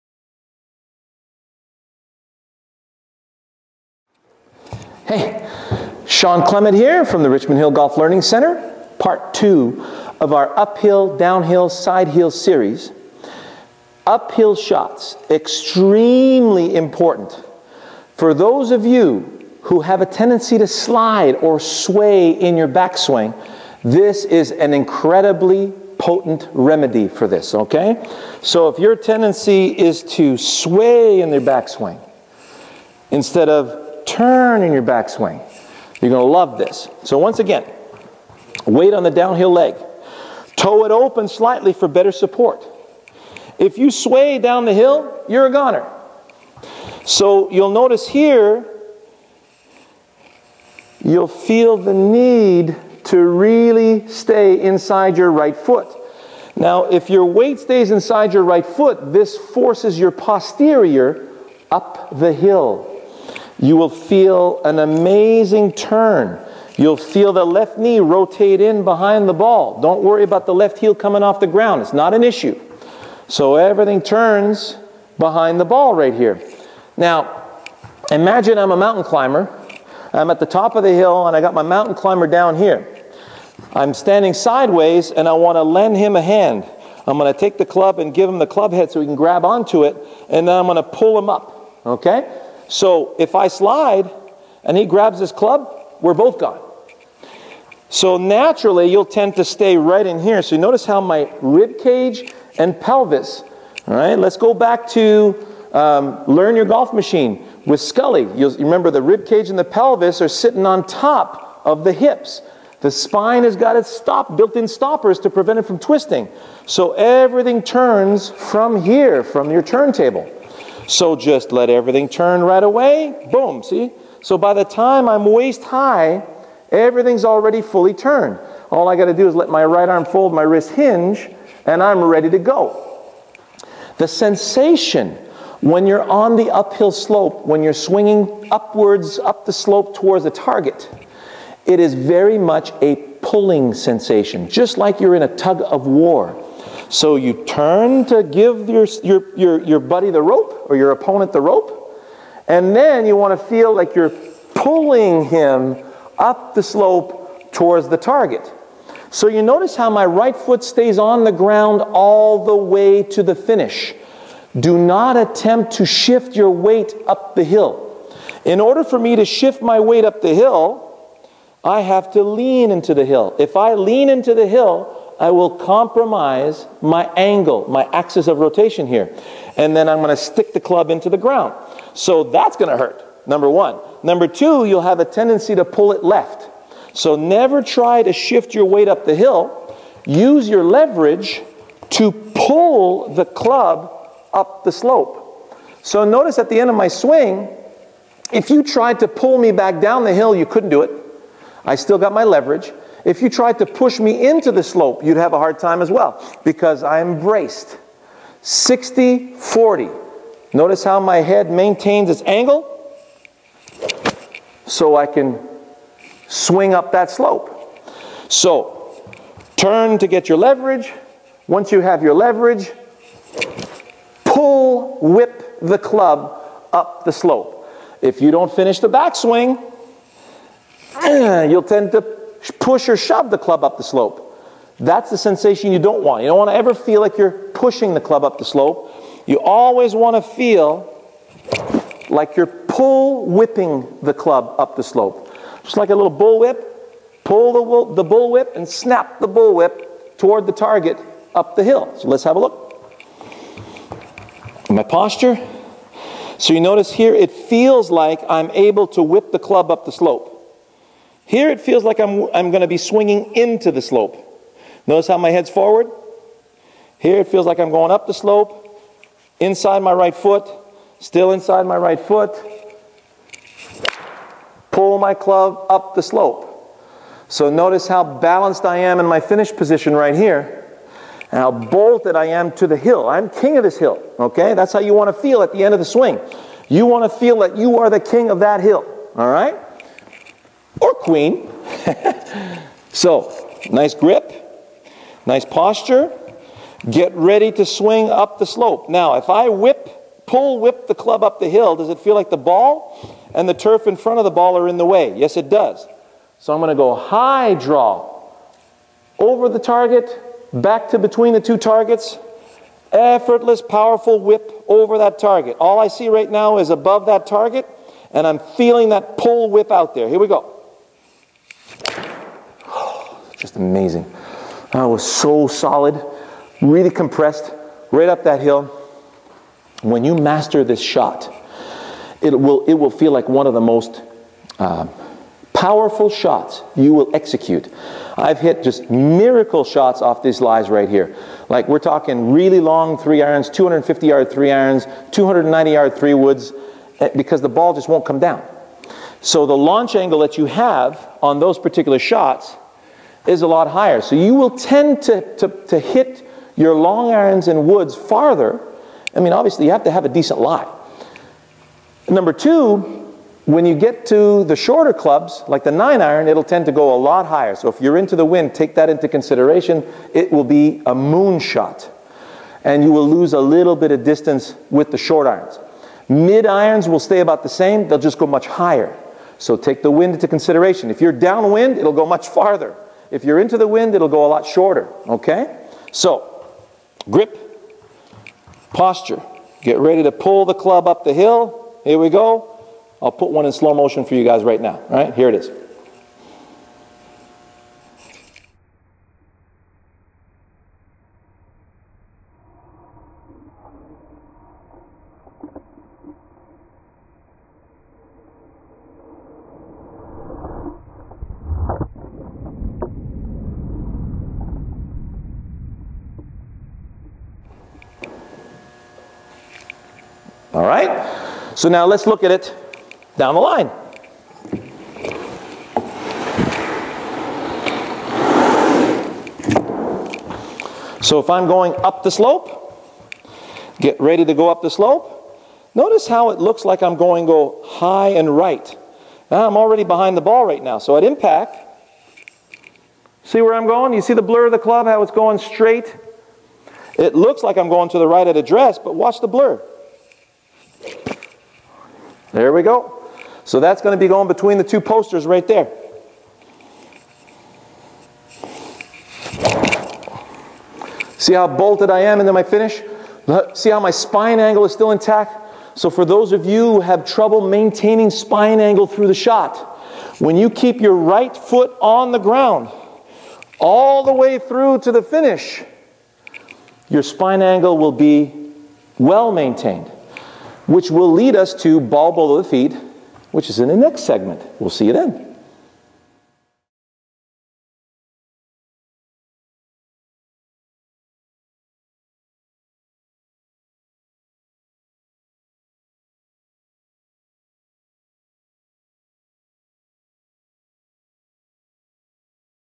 Uphill Shots; Shawn Clement; #1 Most Popular Golf Teacher on You Tube
Shawn Clement, Director of the Richmond Hill Golf Learning Centre, one of the Canadian PGA Top Teachers and Most Popular Teacher-Instructor on You Tube shows you how using the slopes helps refine the golf swing and prevent the dreaded Over the Top and bad weightshift.